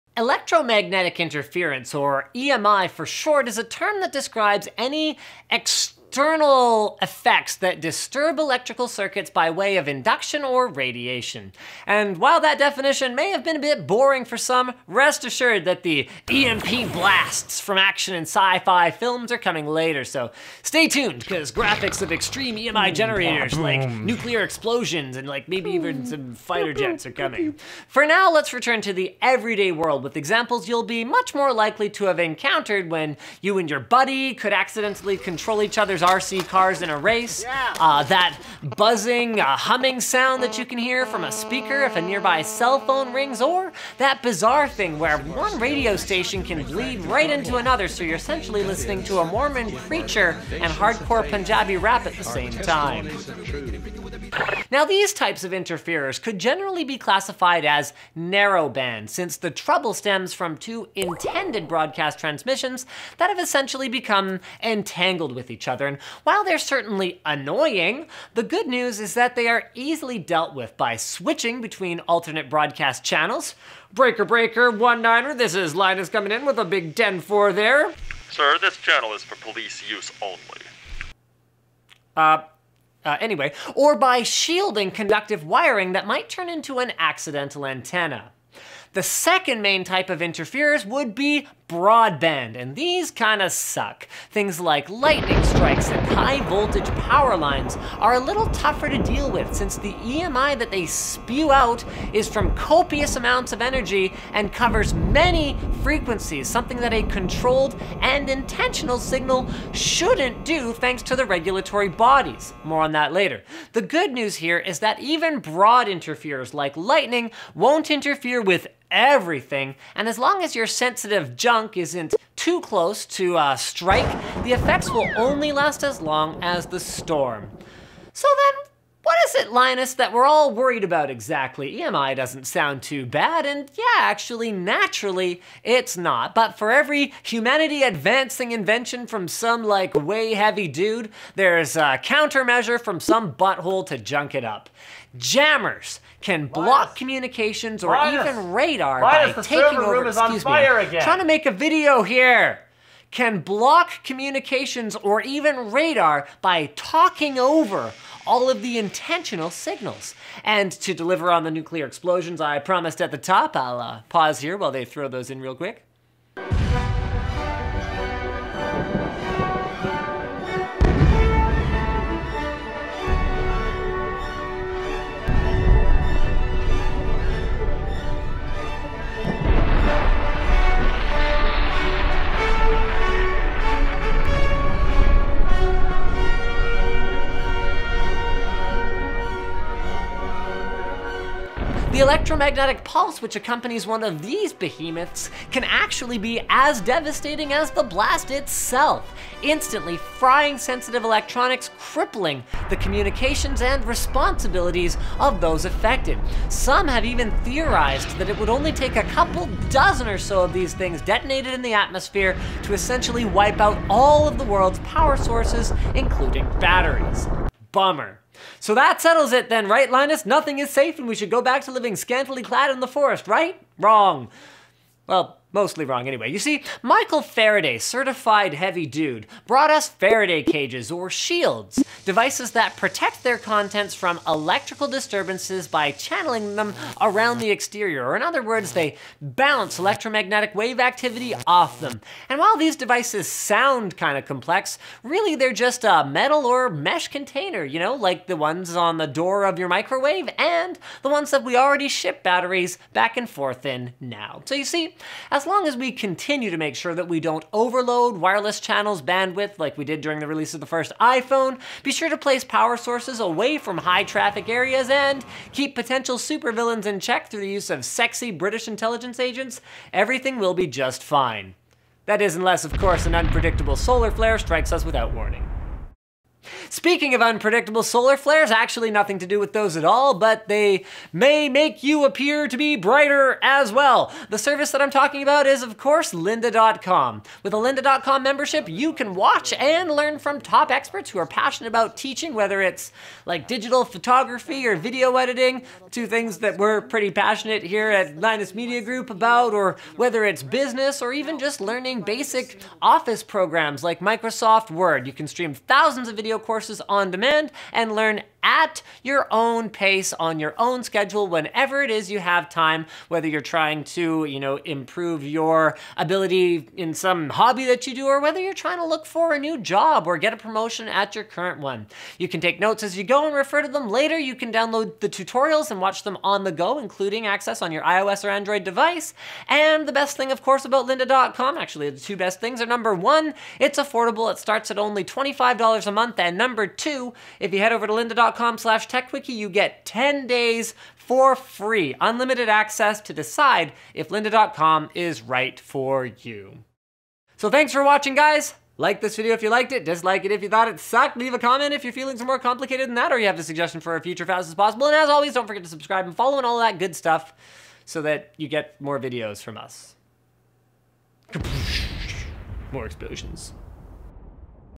Electromagnetic Interference as Fast As Possible
Could a significant electromagnetic interference impact our daily, tech-filled lives as we know them?

IMAGE CREDITS

“Heimbach - power plant 07 ies” by Frank Vincentz - Own work.